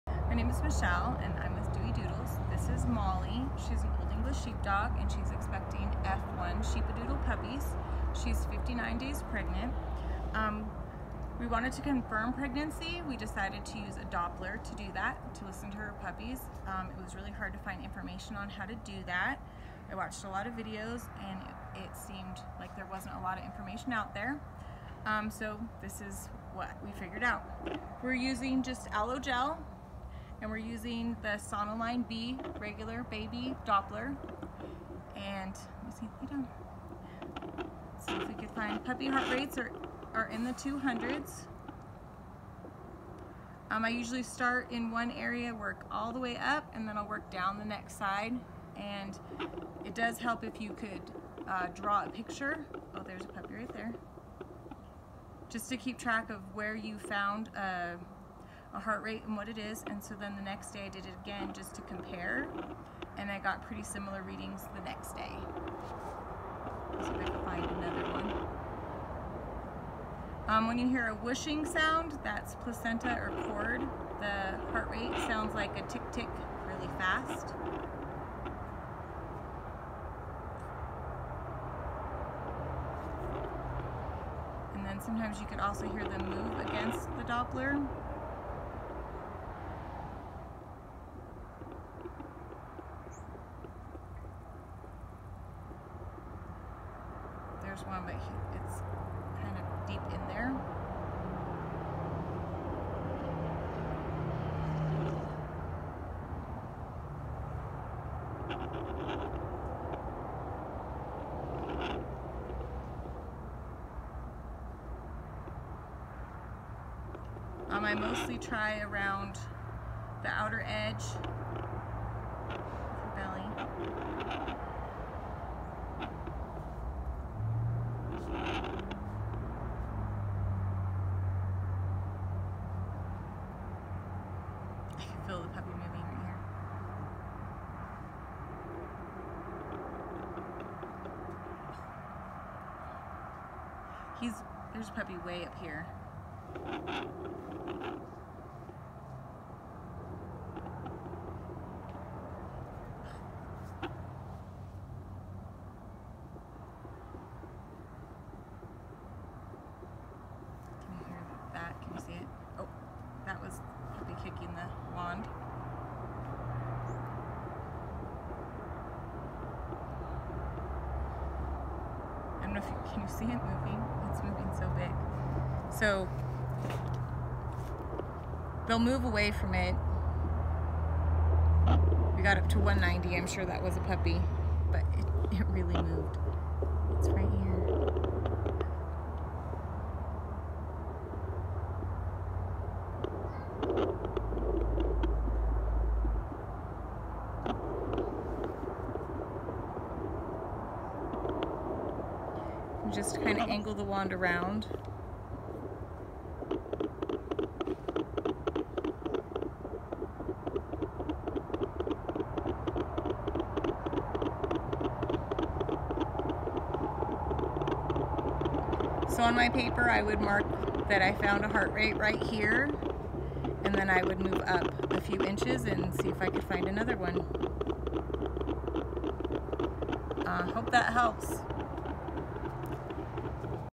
How to use a fetal Doppler on a dog
How to use a Doppler to confirm pregnancy in dogs